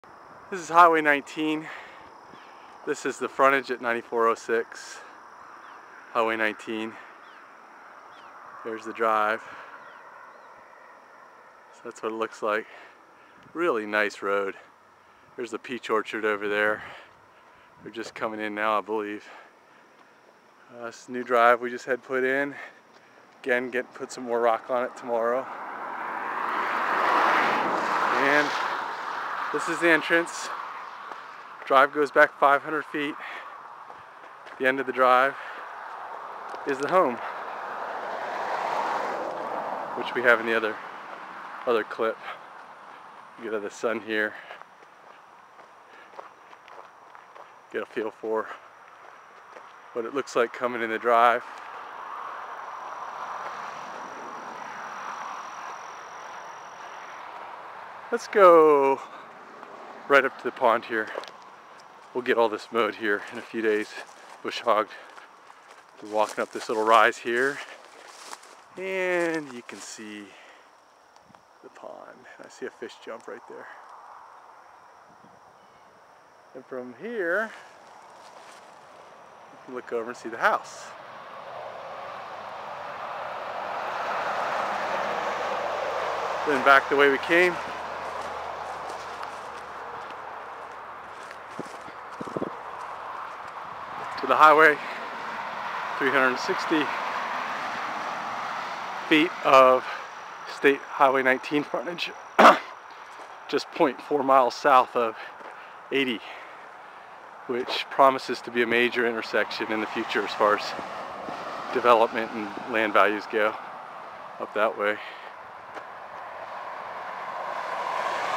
Home on acreage in Edgewood TX
SOLD --  This 3 bedroom, 2 bath mobile home was built in 2005 and sits up on a small hill overlooking 3.72 acres on State Highway 19 north of Canton and just south of Hwy 80. It takes just 6-7 minutes to get to I-20. It is in the Edgewood school district.

The home features open living with 1216 square feet. It is a 2005 single wide mobile home measuring 76'x16'. It has new wood laminate flooring and is ready for you to move into.

The acreage has plenty of trees, a pond and even a year round creek on it. It even has a beaver dam. There is plenty of other wildlife, too.

Though the property has 359 feet of Hwy 19 frontage, the home sets back about 500 feet from the road.